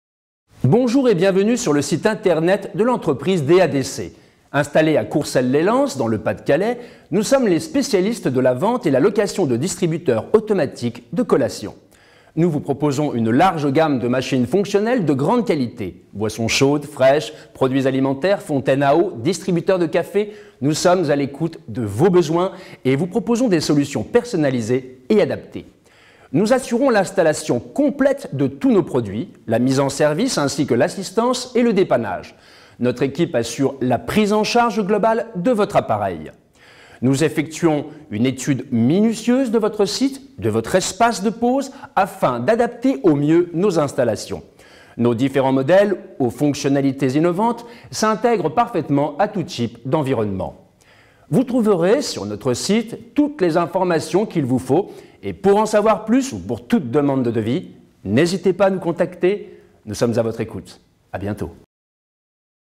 Distributeur automatique de collations : DELFORGE CHRISTIAN - D.AD..C
Bonjour et bienvenue sur le site internet de l'entreprise D.A.D.C.Installés à Courcelles-les-Lens dans le Pas-de-Calais, nous sommes les spécialistes de la vente et la location de distributeurs automatiques de collations.
Nous vous proposons une large gamme de machines fonctionnelles de grande qualité. Boissons chaudes, fraîches, produits alimentaires, fontaine à eau, distributeurs de café... nous sommes à l'écoute de vos besoins et vous proposons des solutions personnalisées et adaptées.
Nous assurons l'installation complète de tous nos produits, la mise en service ainsi que l'assistance et la dépannage. Notre équipe assure la prise en charge globale de votre appareil.
Nous effectuons une étude minutieuse de votre site, de votre espace de pause afin d'adapter au mieux nos installations. Nos différents modèles aux fonctionnalités innovantes s'intègrent parfaitement à tout type d'environnement.
Vous trouverez sur notre site toutes les informations qu'il vous faut. Et pour en savoir plus ou pour toute demande de devis, n'hésitez pas à nous contacter, nous sommes à votre écoute. A bientôt